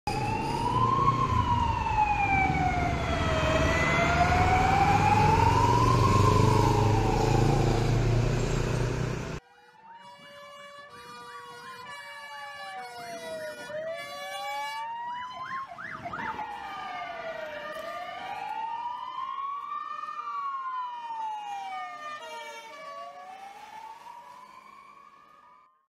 Policía Local de Alicante | Ford Focus CMax
Ford Focus CMax de la Policía Local de Alicante acudiendo a un aviso urgente.